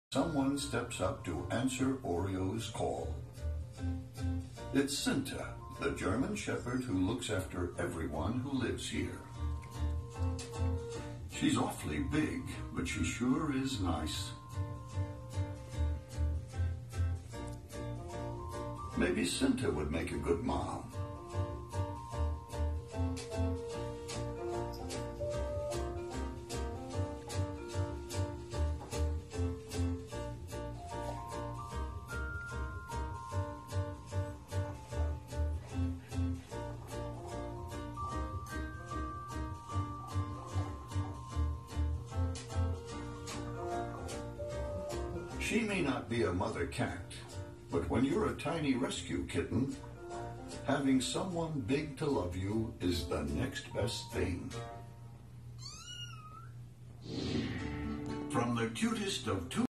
Cat watches German Shepard on TV.
My sidekick KITT joined me on the couch to watch animal planet featuring a German Shepard and a homeless kitten.